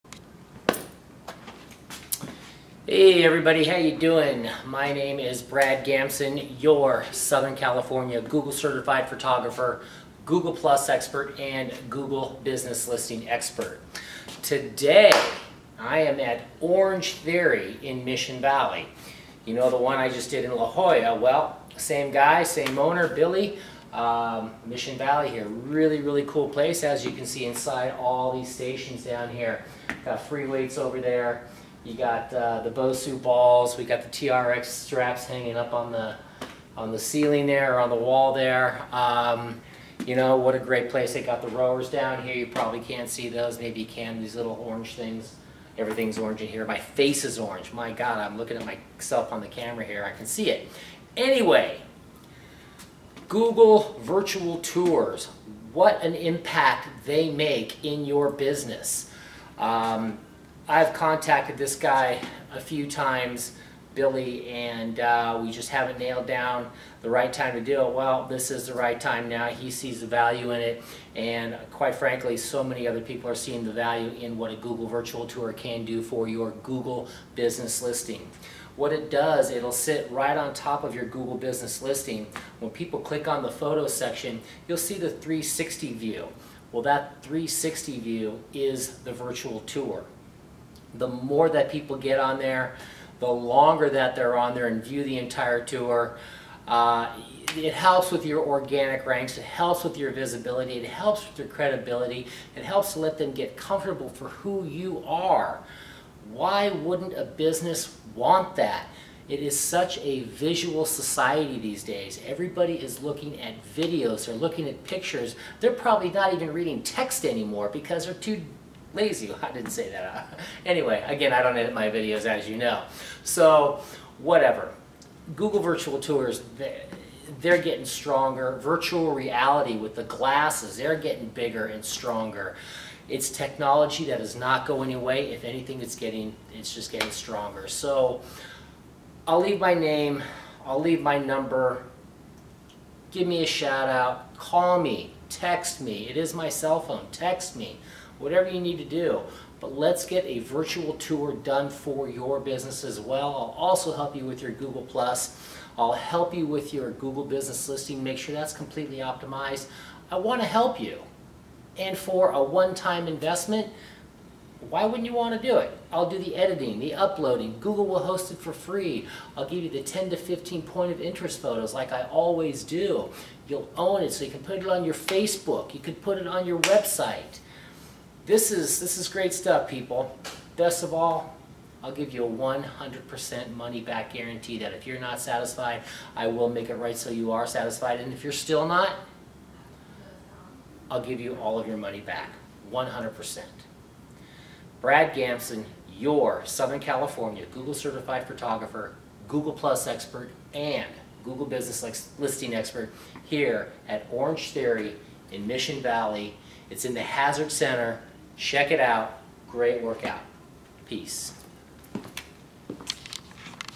Orange Theory Fitness Mission Valley - Google Virtual Tour Photo Shoot.
This is the second one I did for Orange Theory Fitness. Located in Mission Valley, this club has over 500 members. I plan on doing many more Orange Theory clubs in the future as this chain is growing rapidly. Here is the link to the tour.